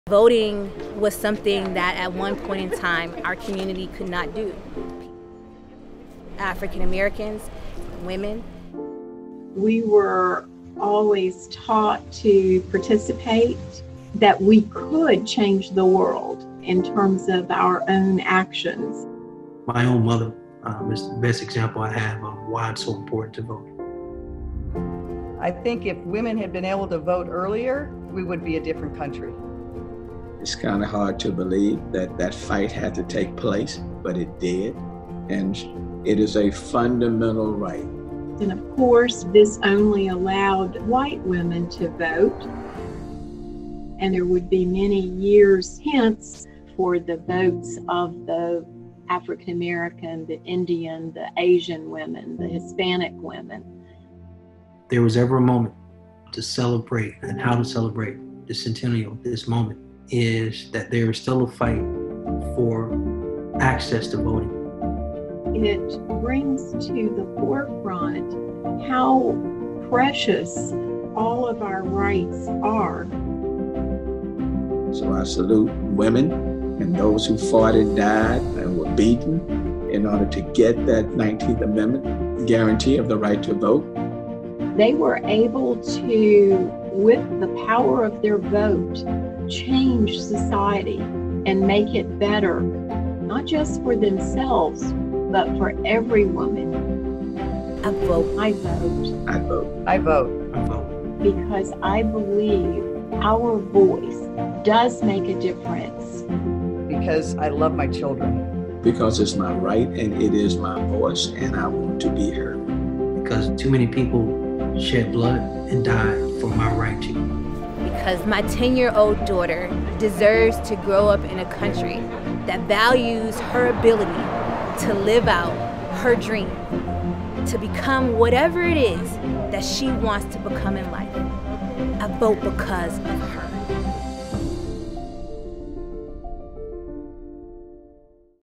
Honoring the Centennial Anniversary of the 19th Amendment | Bloomberg Philanthropies
Today marks the centennial anniversary of the adoption of the Nineteenth Amendment, guaranteeing women the right to vote. We honor those who fought in the decades-long movement for women’s suffrage, and those who fought until all women, regardless of race, could make their voice heard at the ballot box. Hear from mayors across the country who reflect on this moment, and continue to fight to protect voter rights.